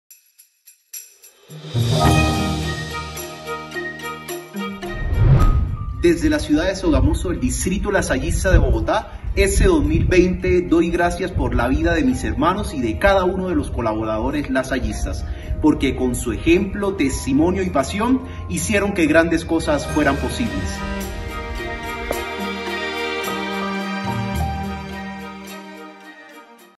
Adviento2020🎄 | Sogamoso
¡En nuestro tiempo de adviento los lasallistas agradecemos por el año 2020!🌟🎉

Producción Gráfica
Gina Jaramillo
Producción audiovisual